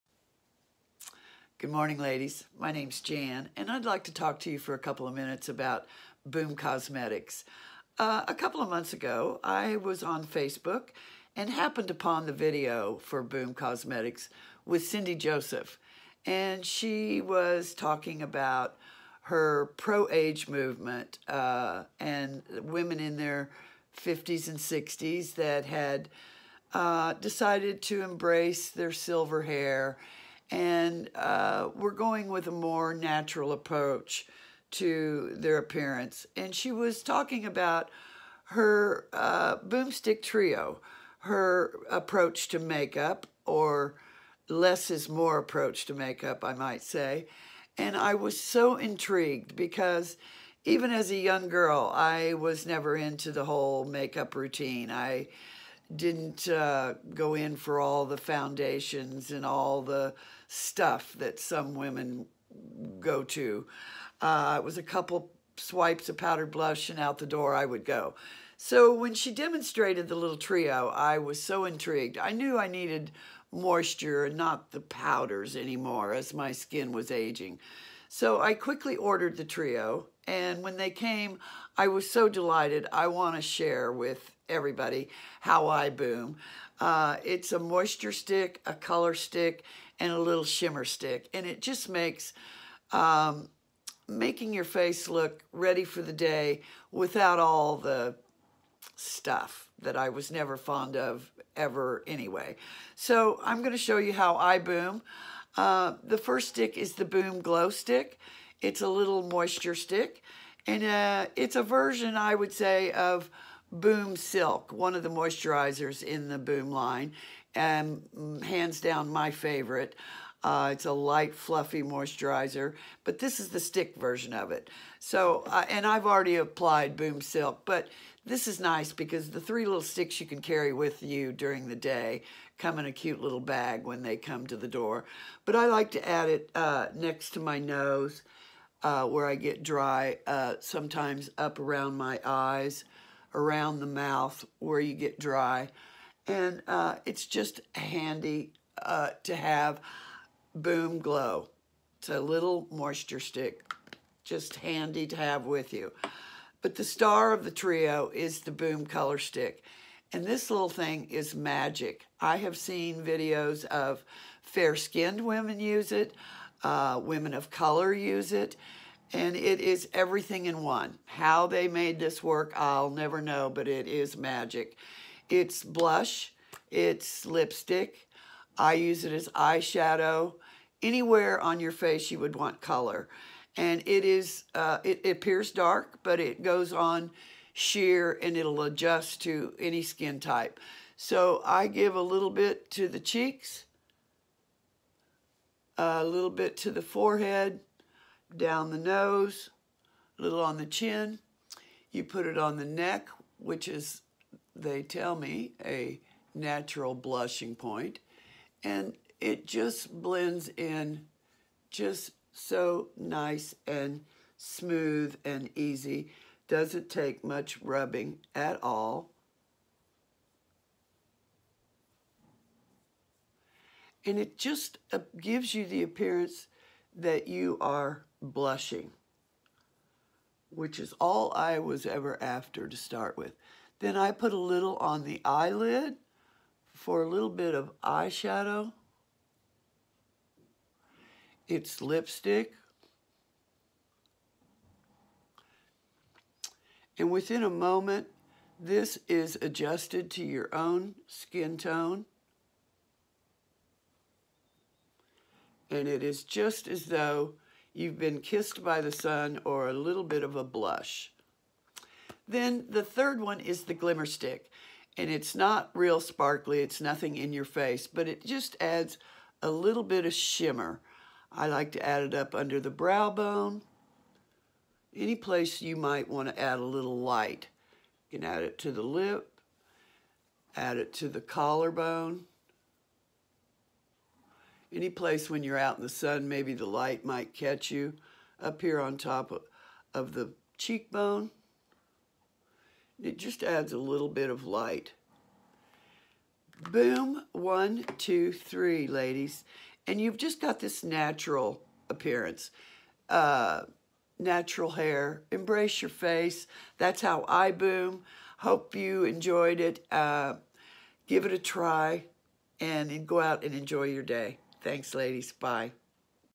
No-Fuss Makeup with Boom Ambassador Jan Plough
Boom Ambassador Jan Plough prefers a no-fuss look. In this video, Jan shares her simple, three-step makeup routine using the Boomstick Trio.